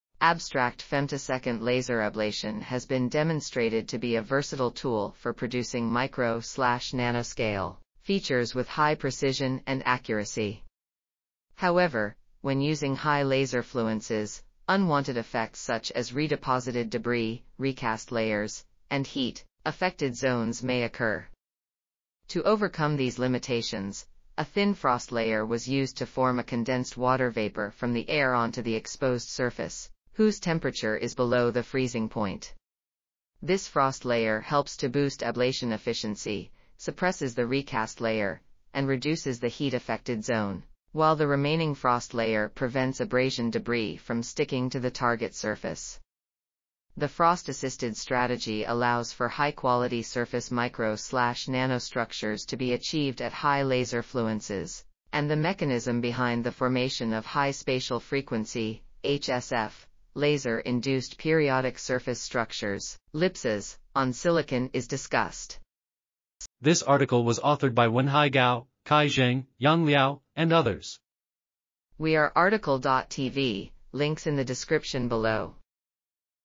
High‐Quality Femtosecond Laser Surface Micro/Nano‐Structuring Assisted by A Thin Fros... | RTCL.TV
### Keywords ###

### Article Attribution ###
Title: High‐Quality Femtosecond Laser Surface Micro/Nano‐Structuring Assisted by A Thin Frost Layer
Authors: Wenhai Gao, Kai Zheng, Yang Liao, Henglei Du, Chengpu Liu, Chengrun Ye, Ke Liu, Shaoming Xie, Cong Chen, Junchi Chen, Yujie Peng ,and Yuxin Leng
Publisher: Wiley-VCH
DOI: 10.1002/admi.202201924

### Video Timestamps ###
0:00:00 - Summary
0:01:06 - Title
0:01:11 - Outro
0:01:16 - End